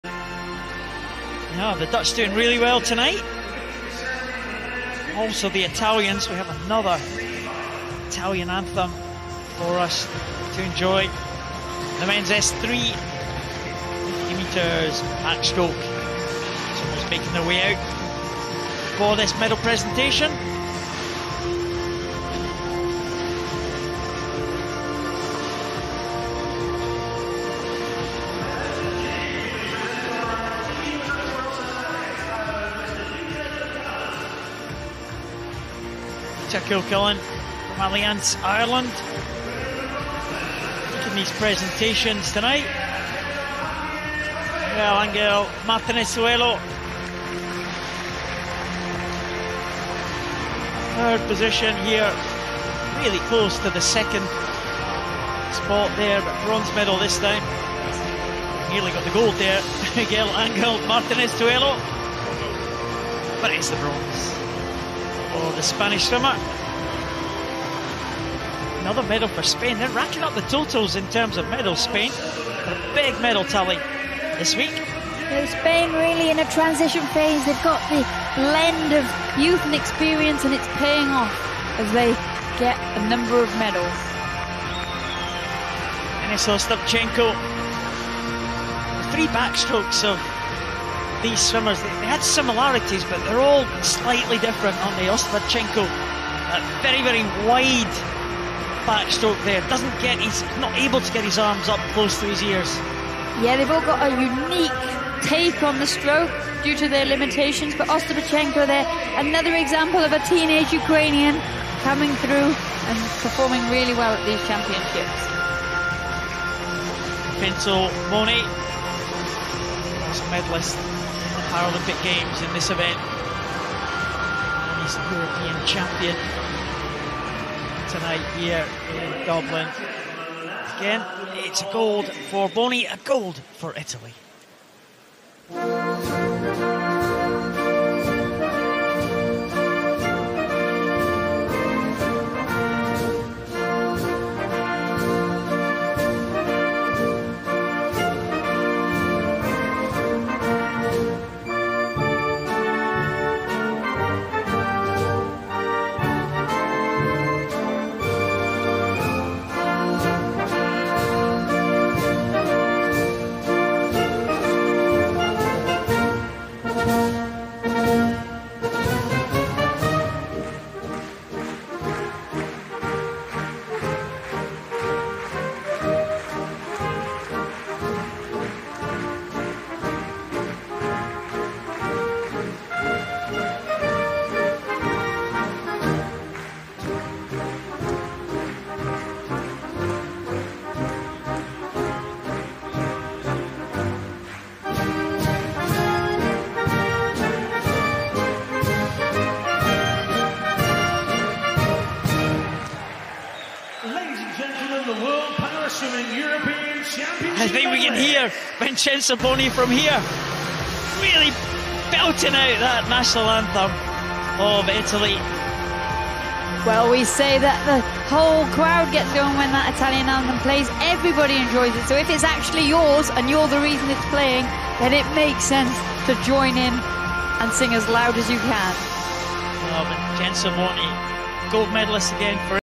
Men's 50m Backstroke S3 Medal Ceremony | Dublin 2018
Medal ceremony of the Men's 50m Backstroke S3 at the 2018 World Para Swimming Allianz European Championships in Dublin, Republic of Ireland.

Results:
GOLD: Vincenzo Boni (ITA)

SILVER: Denys Ostapchenko (UKR)

BRONZE: Miguel Angel Martinez Tajuelo (ESP)